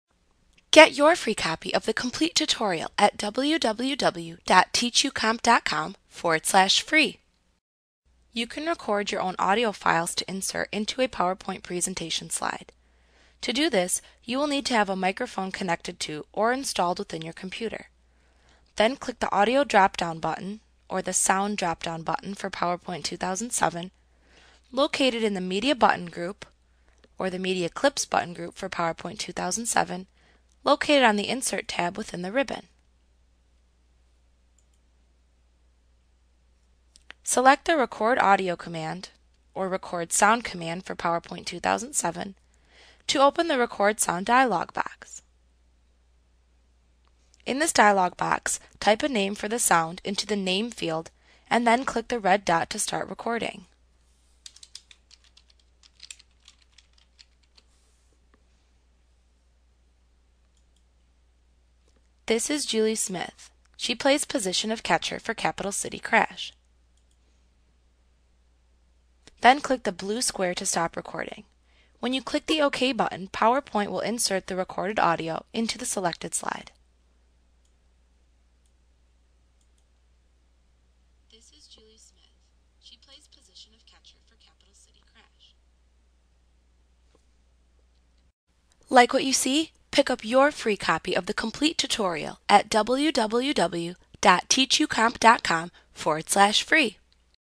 PowerPoint 2013 Tutorial Recording a Sound Microsoft Training Lesson 12.6
Learn how to record a sound in Microsoft PowerPoint - the most comprehensive PowerPoint tutorial available. Visit us today!